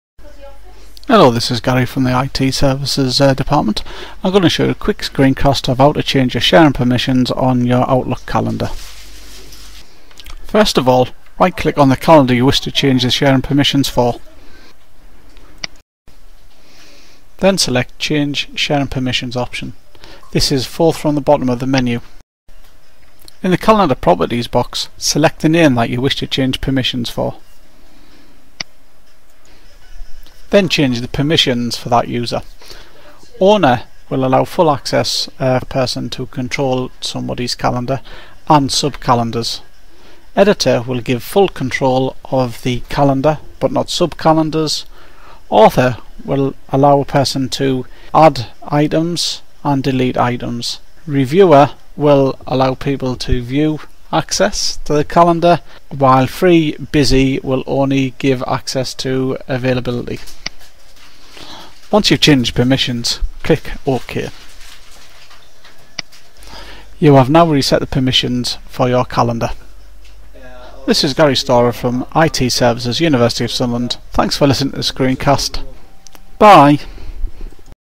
Outlook 2007 Changing your Calendar Permissions (University of Sunderland)
This is screencast that shows you how to change your permissions so others can access your calendar.  The screencast shows you to change permissions using Outlook 2007.